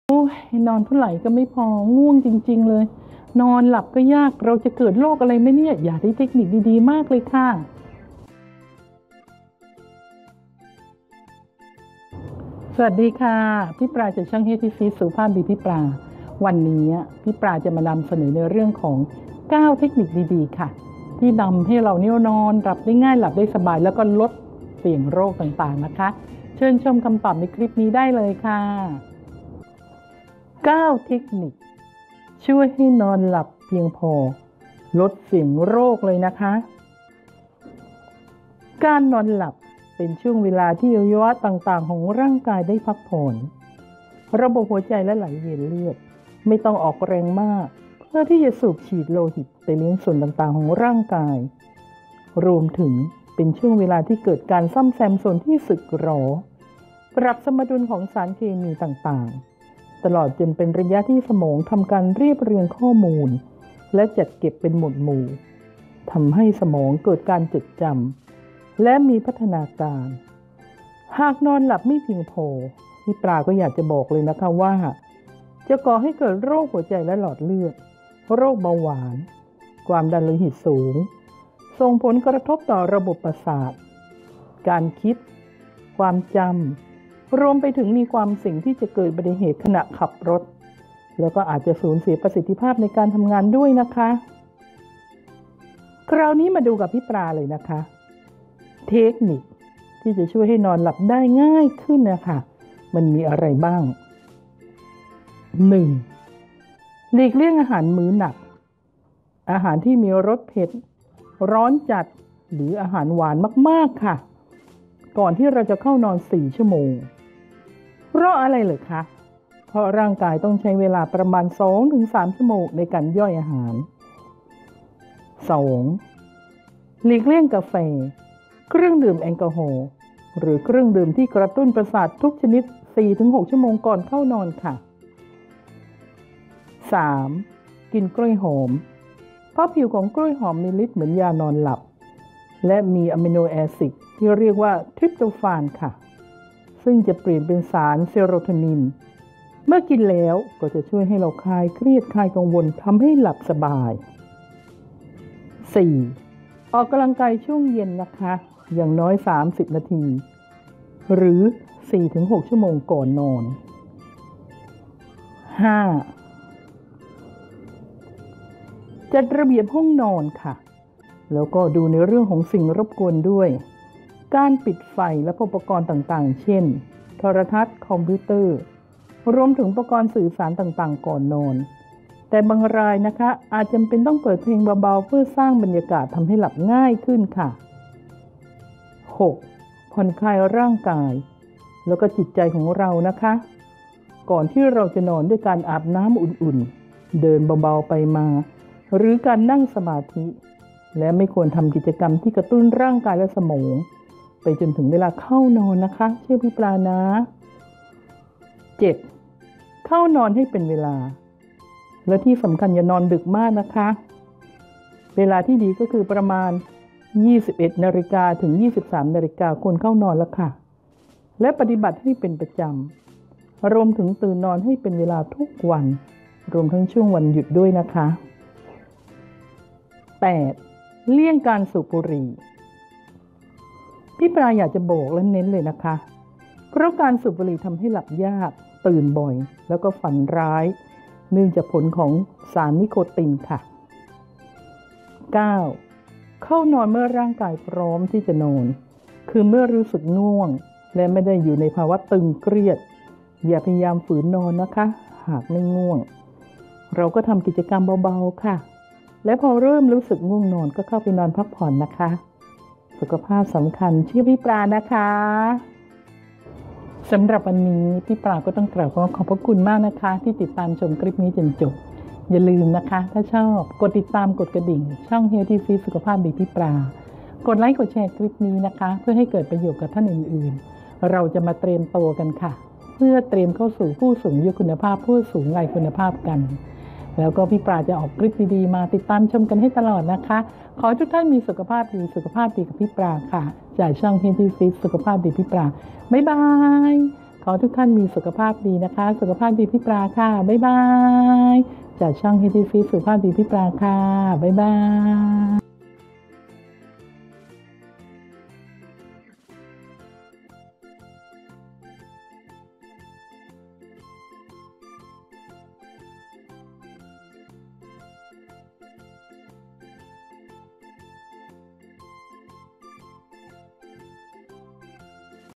9 เทคนิค หลับสบาย ลดโรค | 9 Techniques allow sleep and reduce risk | พี่ปลา Healthy Fish
9 เทคนิค หลับสบาย ลดโรค | 9 Techniques allows sleep and reduce risk
1.หลีกเลี่ยงอาหารมื้อหนัก อาหารที่มีรสเผ็ด รสจัด หรืออาหารหวานมาก ก่อนเข้านอน 4 ชั่วโมง เพราะร่างกายต้องใช้เวลาประมาณ 2-3 ชั่วโมงในการย่อยอาหาร
2.หลีกเลี่ยงกาแฟ เครื่องดื่มแอลกอฮอล์ หรือเครื่องดื่มที่กระตุ้นประสาททุกชนิด 4-6 ชั่วโมงก่อนเวลาเข้านอน
3.กินกล้วยหอม เพราะผิวของกล้วยหอมมีฤทธิ์เหมือนยานอนหลับ และมีอะมิโนแอซิดที่เรียกว่า ทริปโตฟาน ซึ่งจะเปลี่ยนเป็นสารเซโรโทนิน เมื่อกินแล้วจะช่วยคลายเครียด คลายกังวล ทำให้หลับสบาย
4. ออกกำลังกายช่วงเย็นอย่างน้อย 30 นาที หรือ 4-6 ชั่วโมงก่อนนอน
5.จัดระเบียบห้องนอนและกำจัดสิ่งรบกวนด้วยการปิดไฟและอุปกรณ์ เช่น โทรทัศน์ คอมพิวเตอร์ รวมถึงอุปกรณ์สื่อสารต่างๆ ก่อนนอน แต่บางรายอาจจำเป็นต้องเปิดเพลงเบาๆ เพื่อสร้างบรรยากาศทำให้หลับสบายขึ้น
6.ผ่อนคลายร่างกาย และจิตใจก่อนนอนด้วยการอาบน้ำอุ่น เดินเบาๆ ไปมา หรือการนั่งสมาธิ และไม่ควรทำกิจกรรมที่กระตุ้นร่างกายและสมองไปจนถึงเวลาเข้านอน
7.เข้านอนให้เป็นเวลา ไม่ควรนอนดึกมาก ควรเข้านอนเวลาประมาณ 21.00 – 23.00 น. และปฏิบัติให้เป็นประจำ รวมถึงตื่นนอนให้เป็นเวลาทุกวัน รวมทั้งช่วงวันหยุดด้วย
8.เลี่ยงการสูบบุหรี่เพราะจะทำให้หลับยาก ตื่นบ่อย และฝันร้าย เนื่องจากผลของสารนิโคติน
9. เข้านอนเมื่อร่างกายพร้อมที่จะนอน คือเมื่อรู้สึกง่วง และไม่ได้อยู่ในภาวะตึงเครียด อย่าพยายามฝืนนอน หากไม่ง่วง
9 Techniques allows sleep and reduce risk
1. Avoid heavy meals
2. Avoid coffee, alcohol
3. Eat banana
4. Excersie
5. Organize the bedroom
6. Relax mind
7. Sleep not very late
8. Avoid smoking
9. Sleep when the body is ready